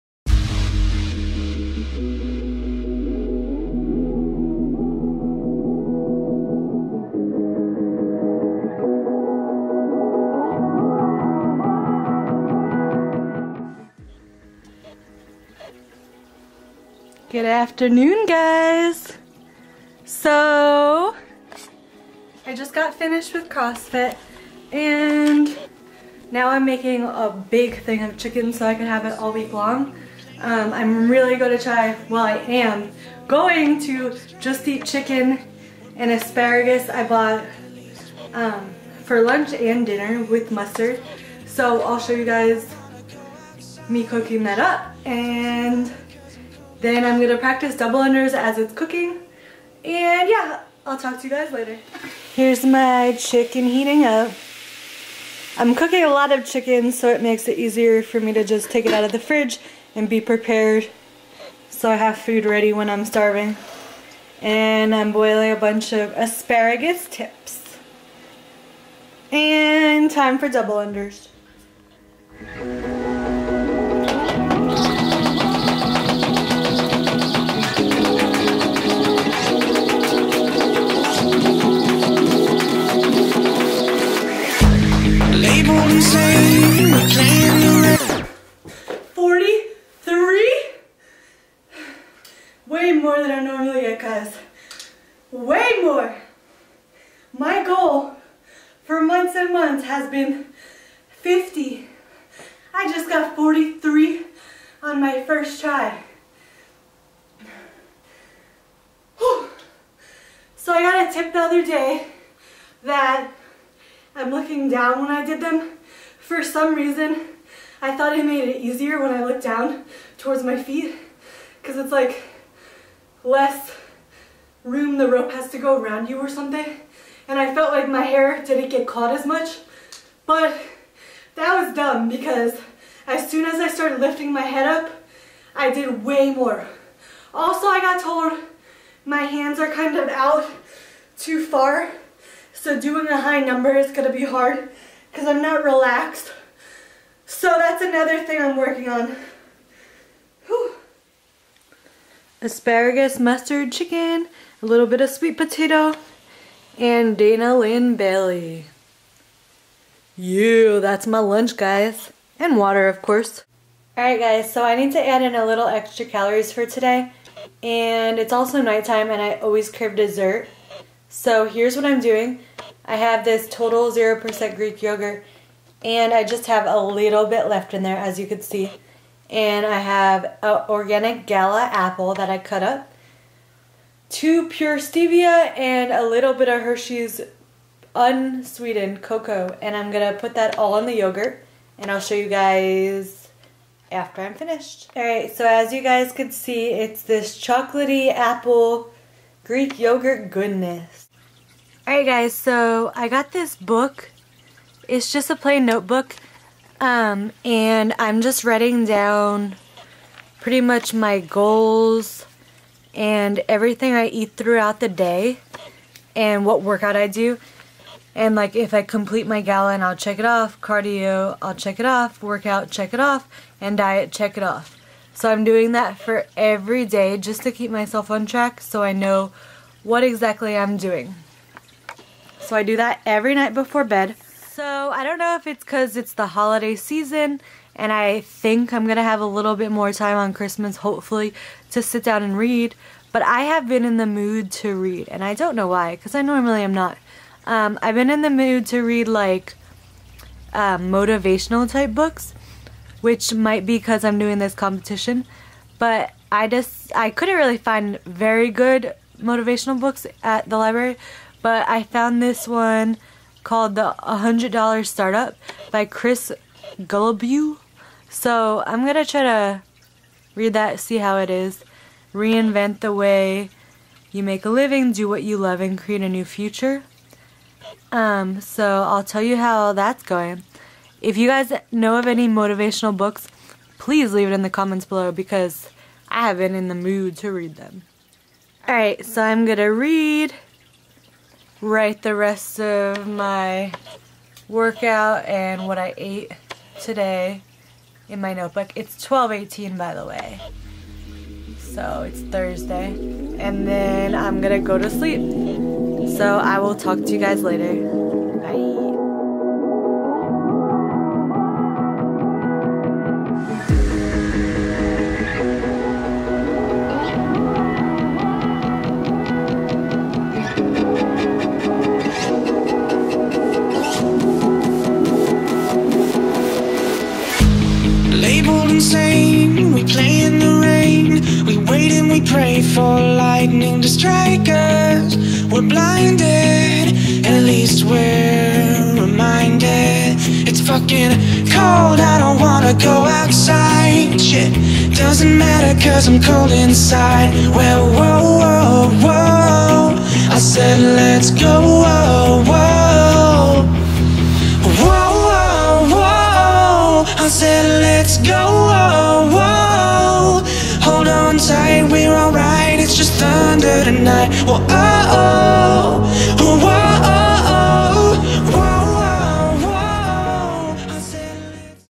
Meal Prep, Double Unders, Inspirational Books, Meal Logging, and Posing- vlog week 4
In this video you will be seeing meal prepping of chicken and asparagus. I finally completed 43 double unders in a row and gave you some tips I learned. I checked out a motivational book out of the library. I also show you how I keep track of my meals and workouts. Last but not least if you get it to the end of the video you see my posing. It is hard to get good lighting to pose!